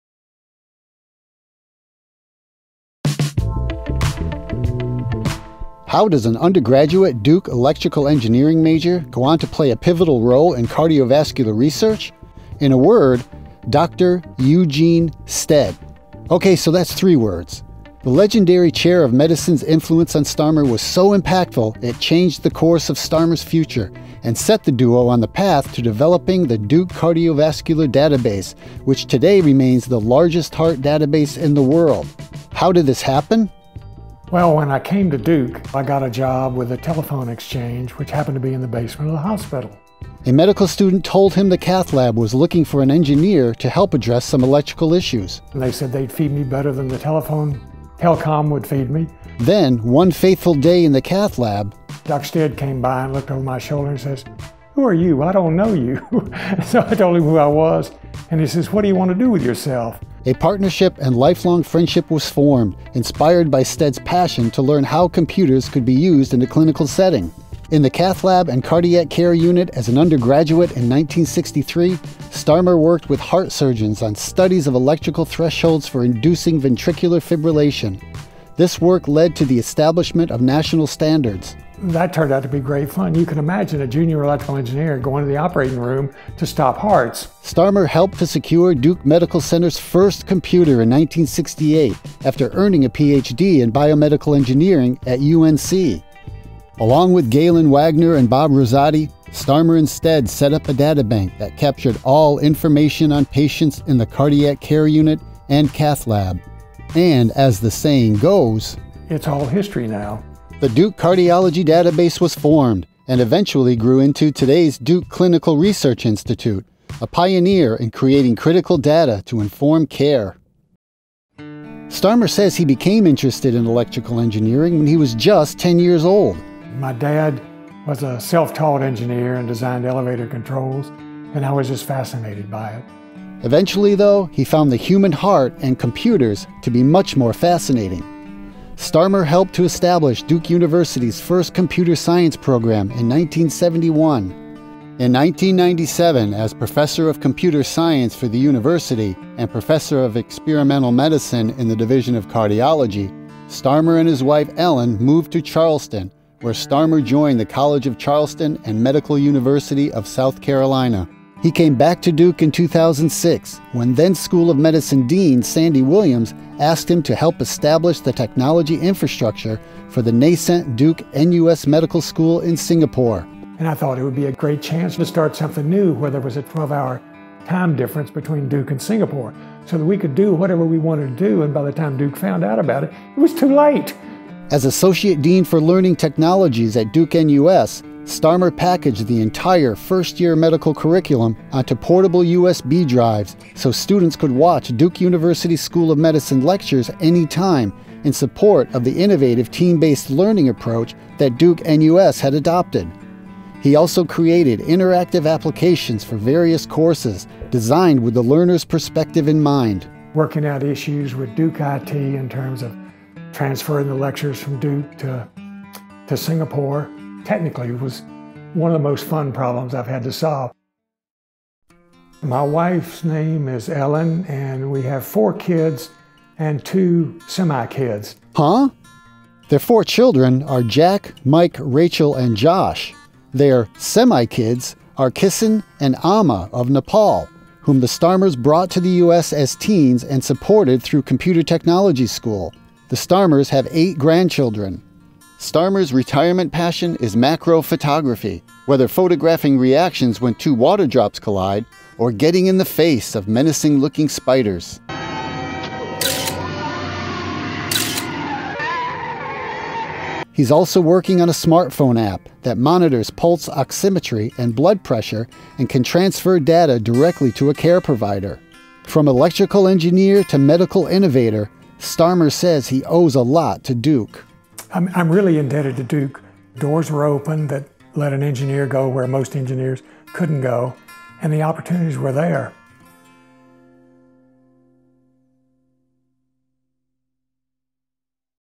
2018 Duke Medical Alumni Awards: C. Frank Starmer, BSEE'63, G'65, PhD
C. Frank Starmer, BSEE'63, G'65, PhD, who began his time at Duke University as an undergraduate electrical engineering major, helped Duke's legendary Chairman of Medicine Eugene Stead, MD, create the Duke Cardiovascular Database, which grew into the largest of its kind in the world. Starmer also played a key role in creating the technology infrastructure for the Duke-NUS Medical School in Singapore. Starmer was honored with a Distinguished Faculty Award during the 2018 Duke Medical Alumni Reunion in November.